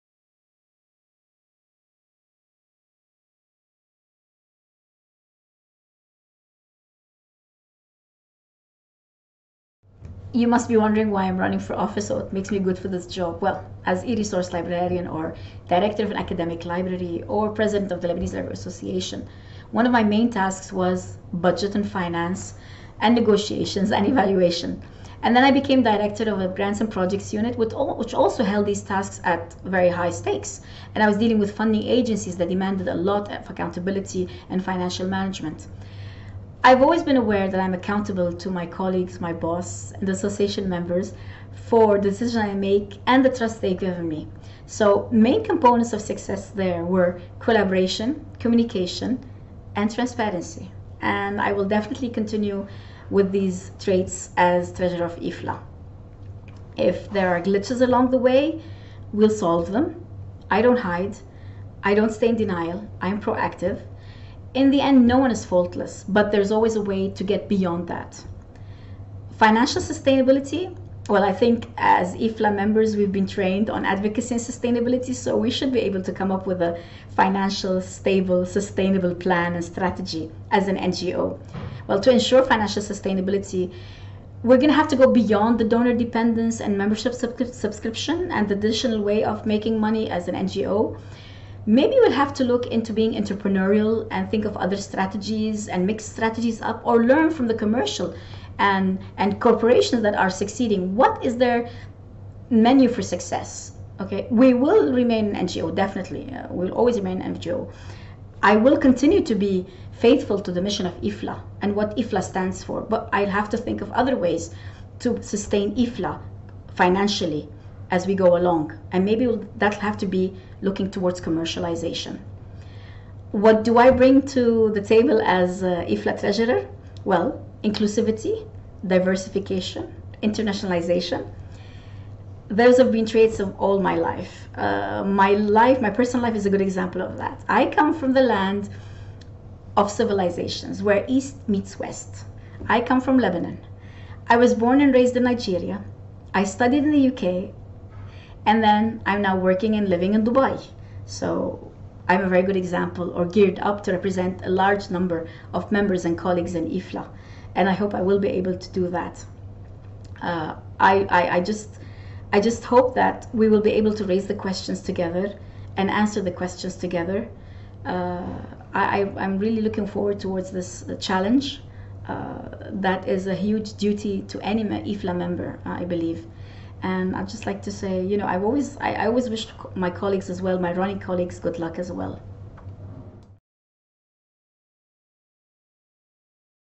IFLA Elections 2023: Treasurer Candidate, Randa al Chidiac
IFLA Elections 2023

Candidate video statement: Treasurer

Members have expressed a high interest in the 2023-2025 IFLA elections and the candidates that have been nominated. At its meeting in February 2023, the Governing Board agreed to ask candidates for the major roles of President-elect, Treasurer and Governing Board member at large to provide a brief video presentation in addition to their motivation statement.

To provide a standard approach, candidates for Treasurer were asked to briefly introduce themselves and speak to the following questions:

*What experience and skills do you bring to the role of IFLA Treasurer?
*As Treasurer, what do you see as the opportunities and challenges for IFLA to be financially sustainable?
*As Treasurer, how do you see your role and responsibility within the Governing Board?